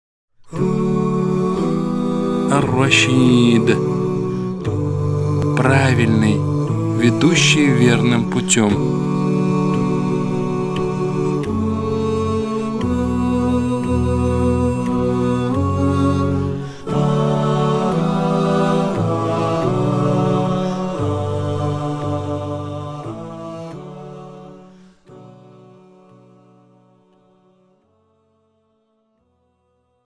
99 имен Аллаха - 98 - Ар-Рашиид | Учим имена Всевышнего - 98
99 имен Всевышнего Аллаха - Серия "Учим имена Всевышнего Аллаха" - 98. Значение имени Ар-Рашиид:
الرشيد -
Правильный, Ведущий верным путем, Разумный.
Направляющий на правильный путь; Тот, Кто даёт счастье тому, кому хочет, направляя его на истинный путь; Тот, Кто отдаляет того, кого хочет, по установленному им порядку.
В Священном Коране имя Ар-Рашиид упоминается в сурах: 2:256; 11:87.